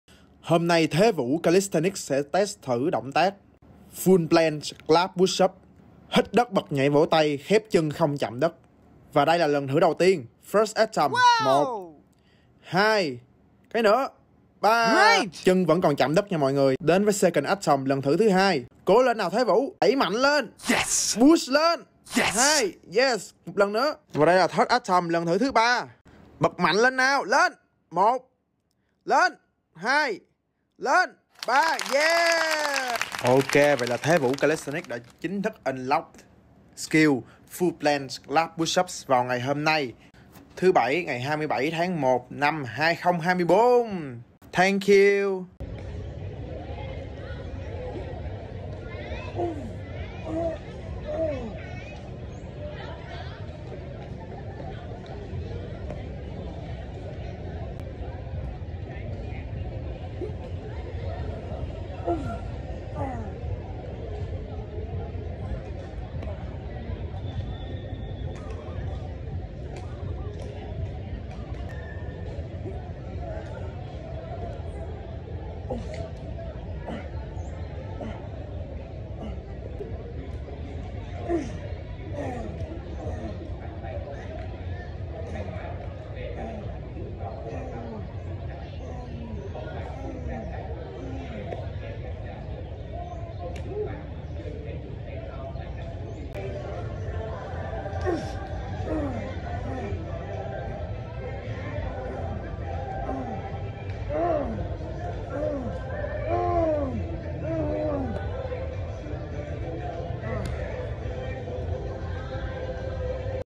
The Vu Calisthenics hít đất vỗ tay Full Planche Clap Push Ups | Lê Phạm Thế Vũ
Thế Vũ thử hít đất nhảy vỗ tay Full Planche Clap Push Up vào sáng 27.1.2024

Calisthenics & Street Workout Athlete from Vietnam - Le Pham The Vu  - 25 years old 💪🏻🇻🇳